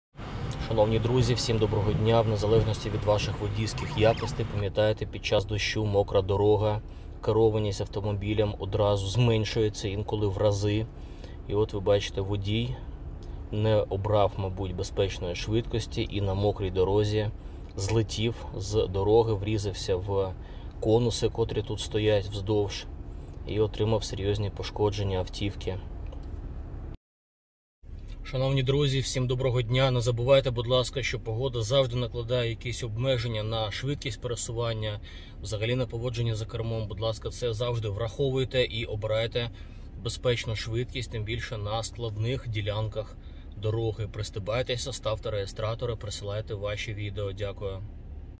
Карколомна автотрощща дтп авария в Коростене: утром на мокрой дороге водитель автомобиля
Карколомна автотрощща дтп авария в Коростене: утром на мокрой дороге водитель автомобиля не справился с управлением и вылетел с дороги , врезавшись в конусы. Автомобиль серьёзно повреждён. Обошлось без пострадавших.

 Призываем граждан вне зависимости от их амбиций и самоуверенности в мокрую погоду снижать скорость, увеличивать дистанцию и вообще быть намного более осторожными чем обычно.

Также присылайте ваши видео, с нас всегда пиар реклама и поддержка!